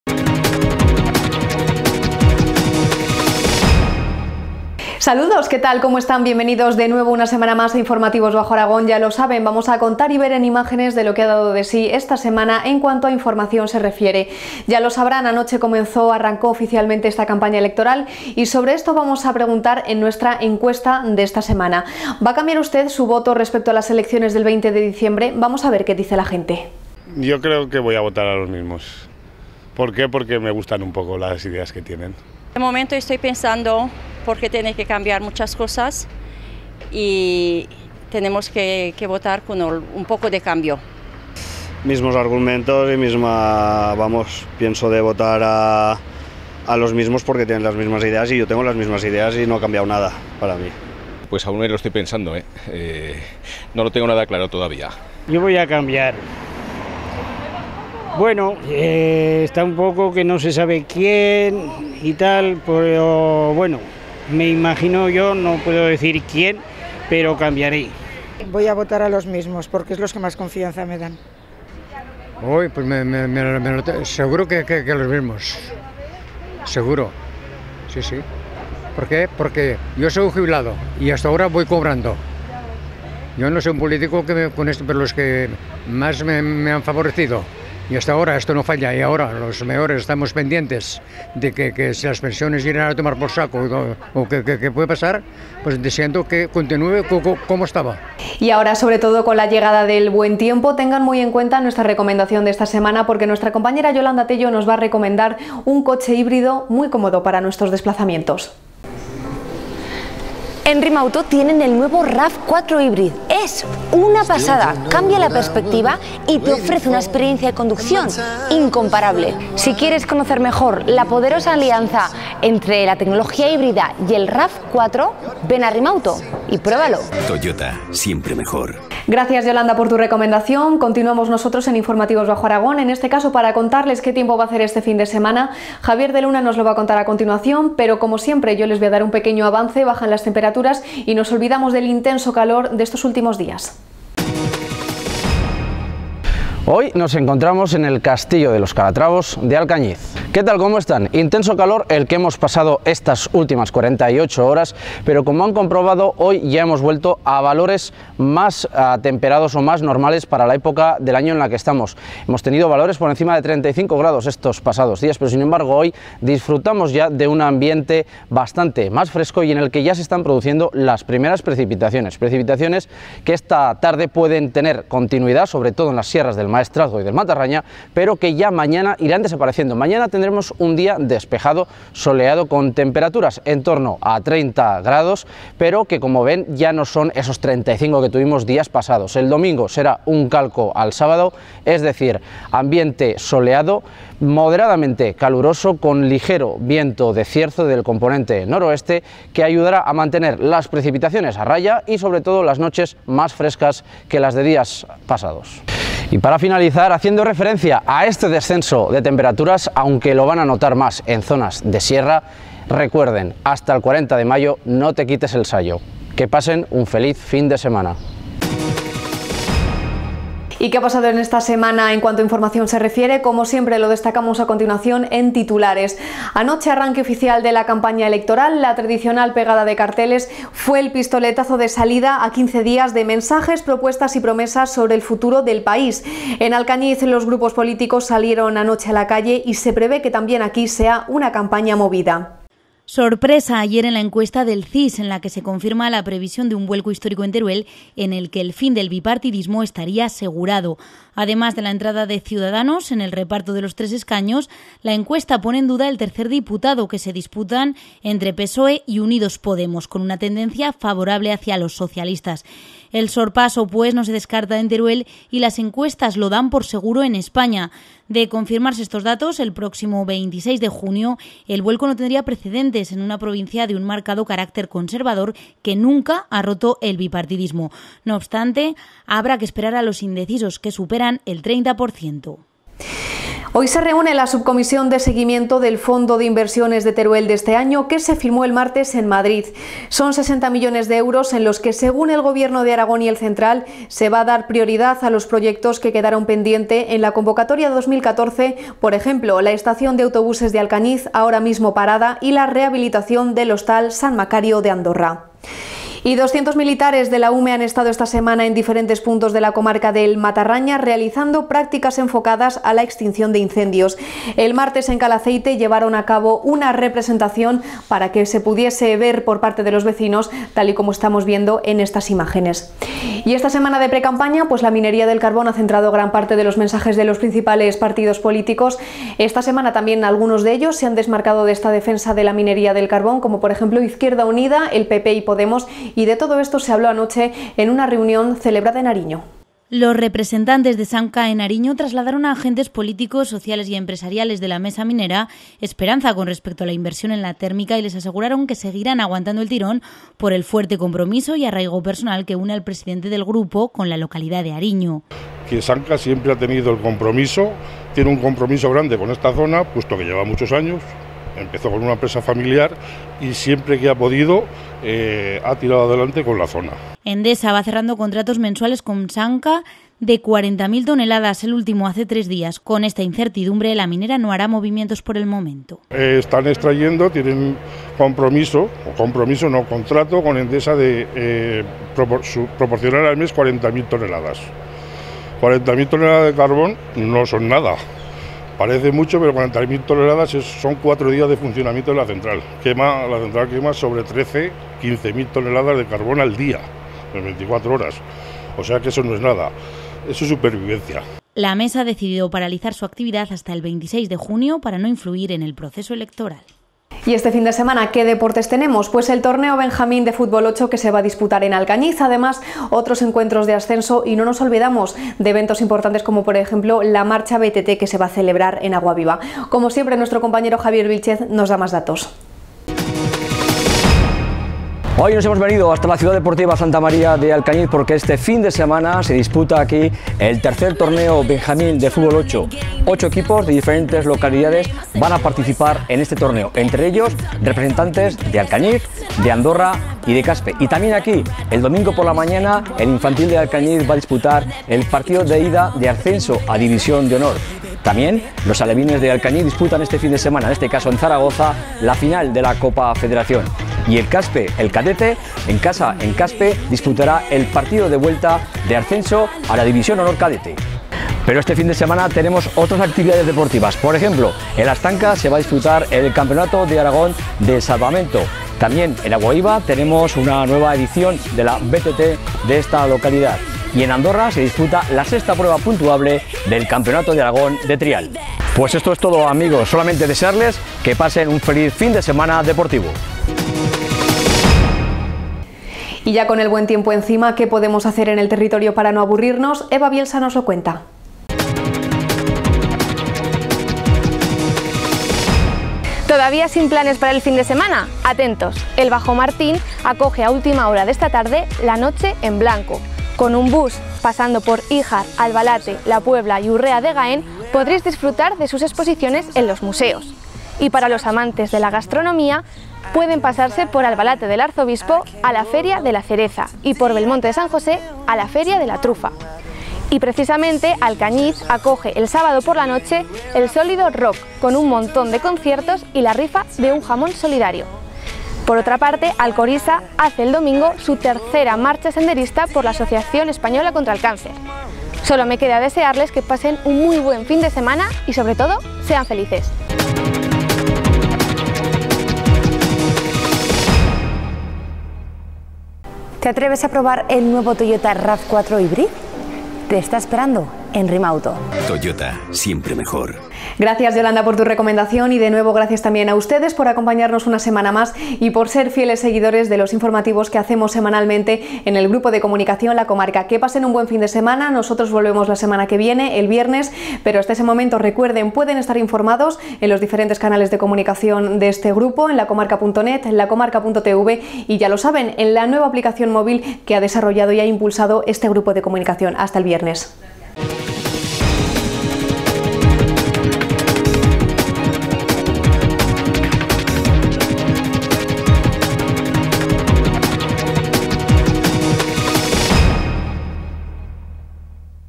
Informativos Bajo Aragón - 10 junio 2016
Arranca la campaña electoral y hemos salido a la calle para ver si el 26-J van a cambiar su voto respecto al 20-D

1. El CIS anuncia el fin del bipartidismo en Teruel y no descarta el ‘sorpasso’
2. Montoro firma los 30 millones del FITE
3. Los 260 militares de la UME abandonan hoy el Matarraña
4. Samca reafirma su compromiso con Ariño y cree que la inversión llegará

Aquí les dejamos con la actualidad más destacada de la semana, más información y